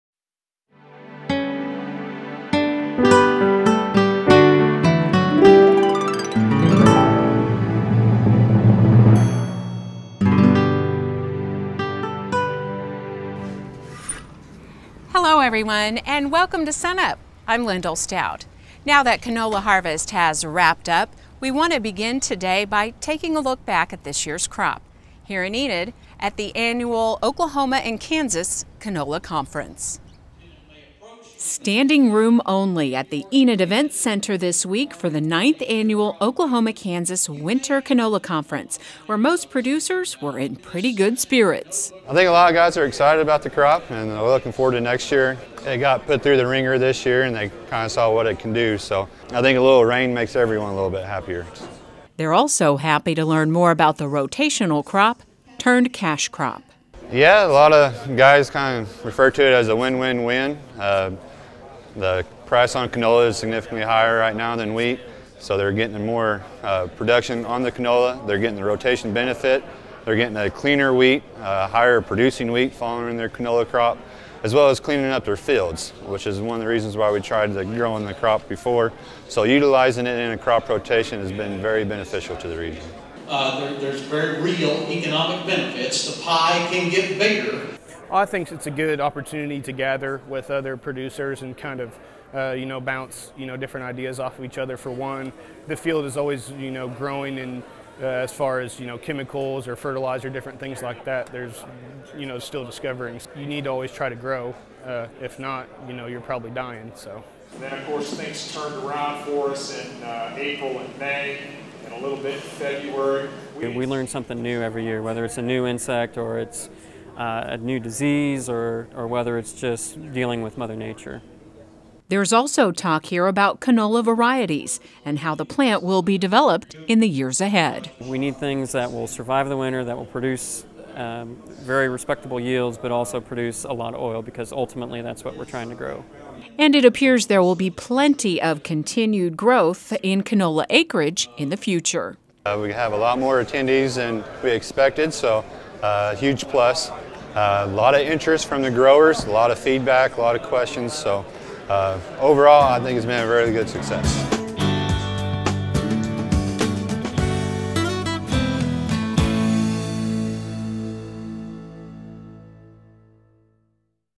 Canola Conference (7/20/13)
We travel to Enid for the 9th Annual Oklahoma-Kansas Winter Canola Conference to talk with Josh Bushong, Mike Stamm and Larin Wiens, a canola producer.